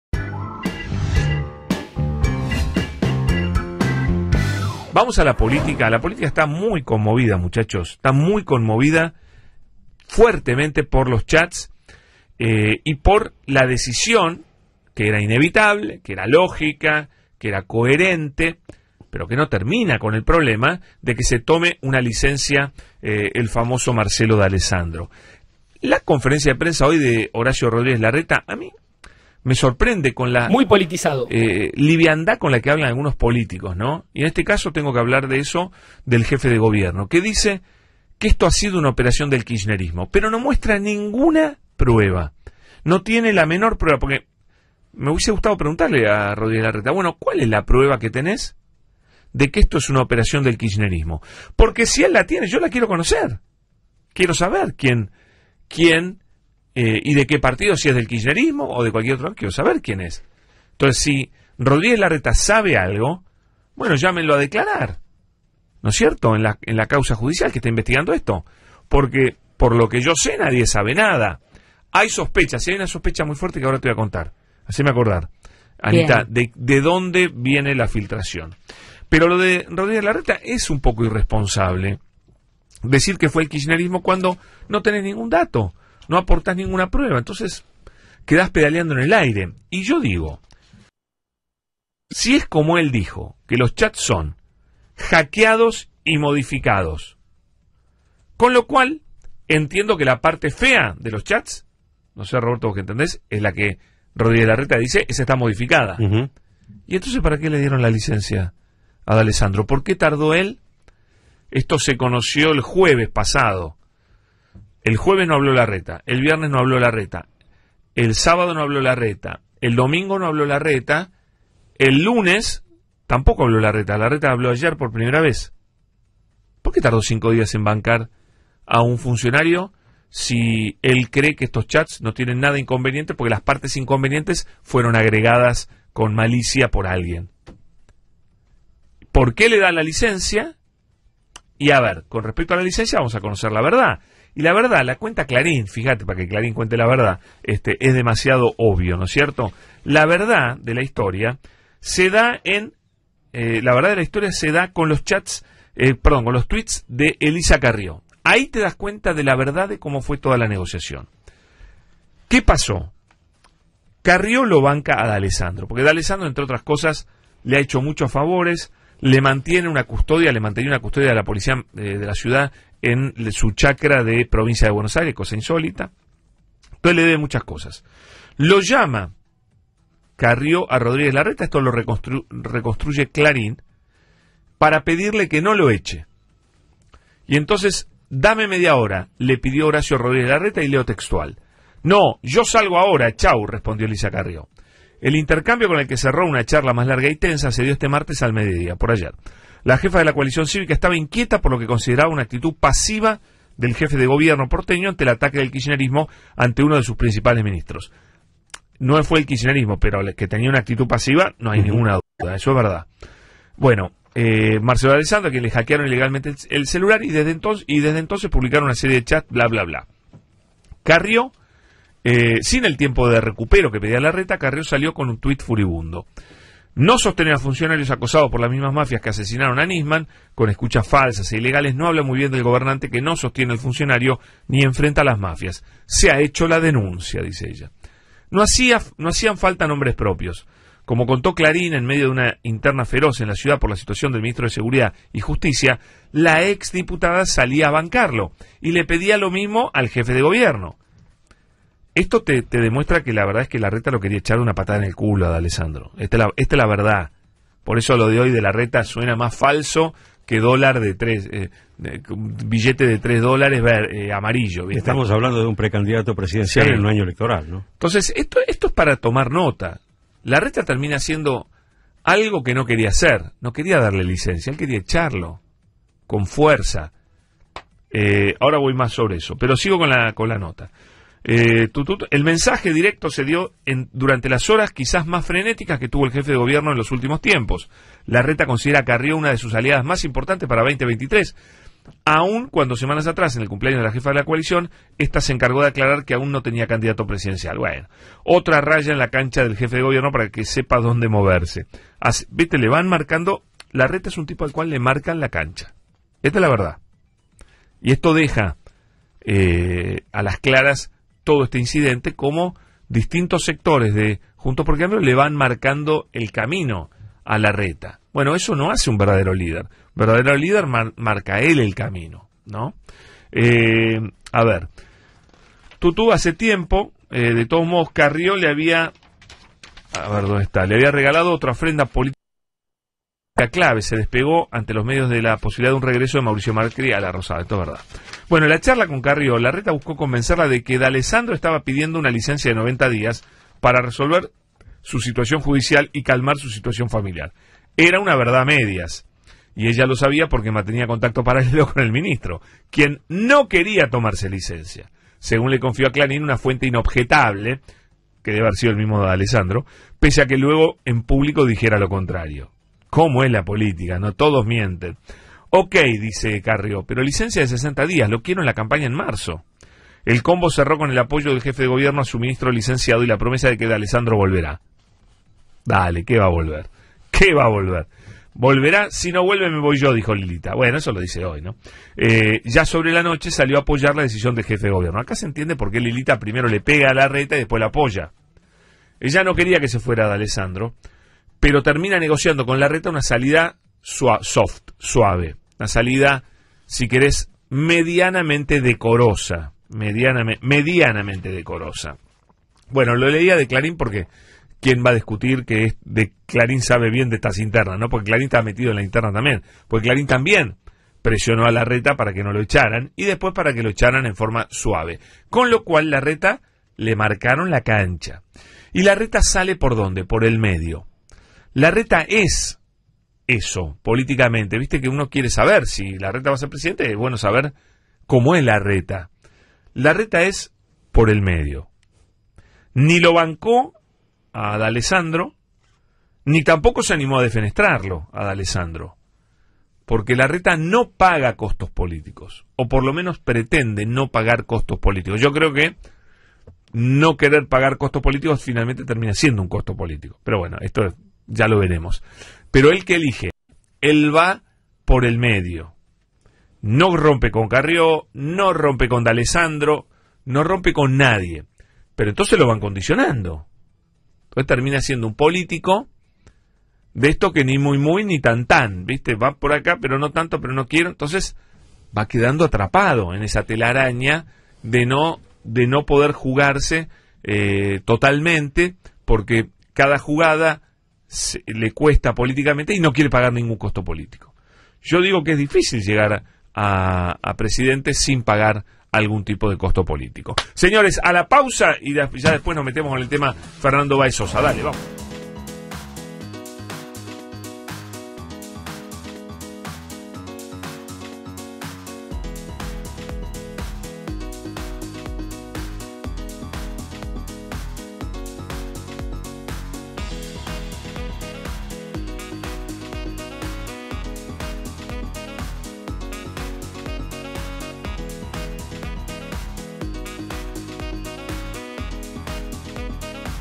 Nuevo capítulo sobre el escándalo de los chats de D'Alessandro: Duggan analiza data sobre Carrió
Nuevo capítulo en el escándalo de los chats del ministro Marcelo D'Alessandro: Pablo Duggan editorializa sobre las últimas publicaciones periodísticas que hunden al ministro de Larreta y a Lilita Carrió. Las internas en Juntos por el Cambio están en su mayor esplendor.
¿Nos das una mano para seguir subiendo videos? Aportes al canal, acá